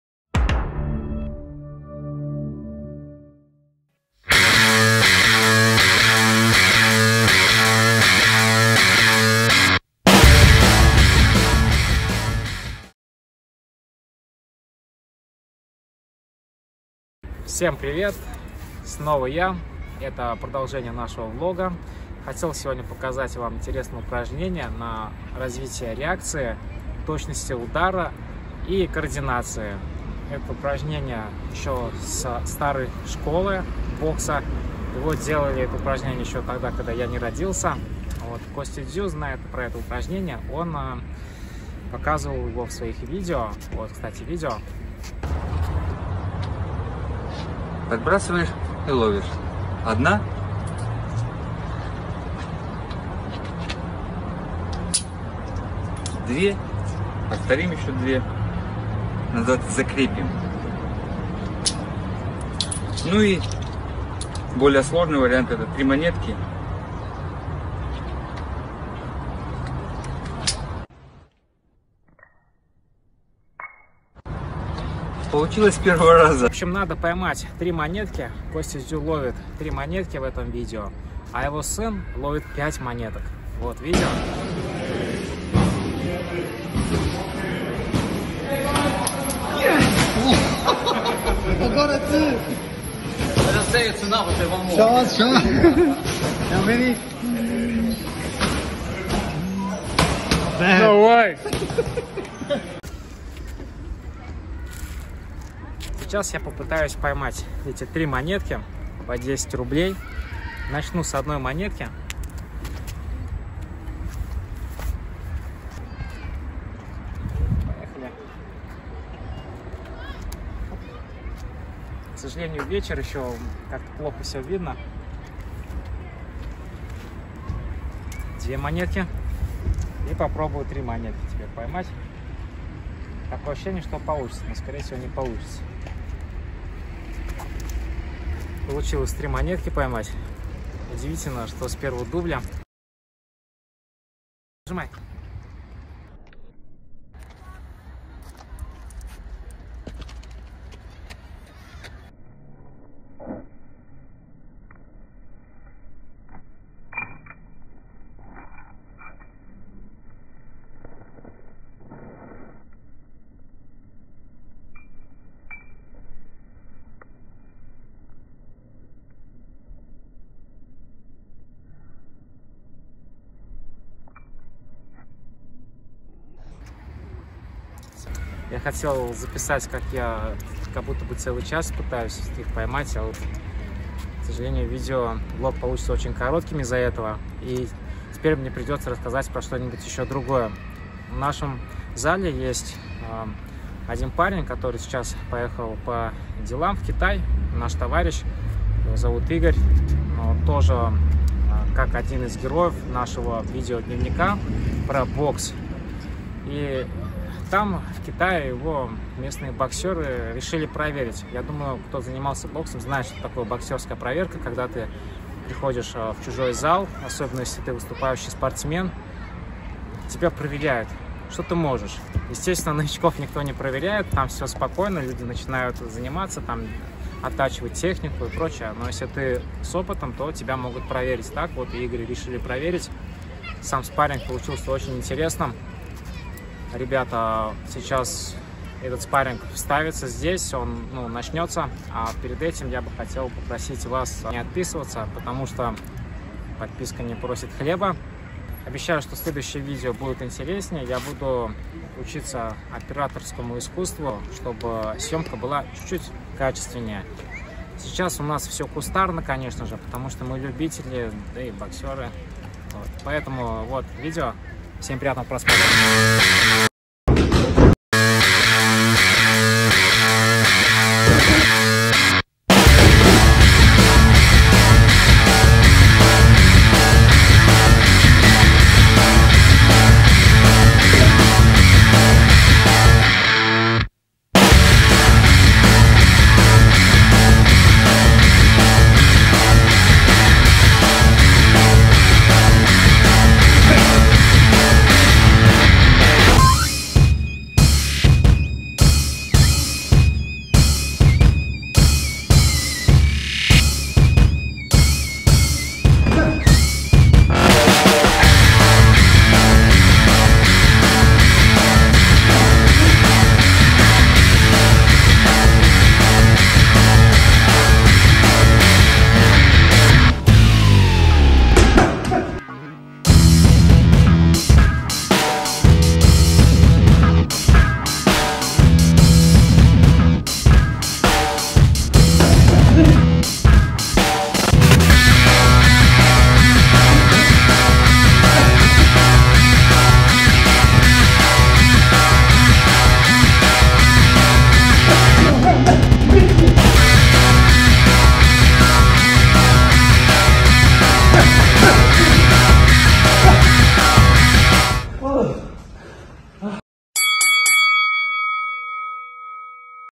Влог про бокс и спорт. Выпуск #3
В третьей части поговорим о том как развить реакцию боксера с помощью старого дедовского способа, о котором все забыли, но мы напомним.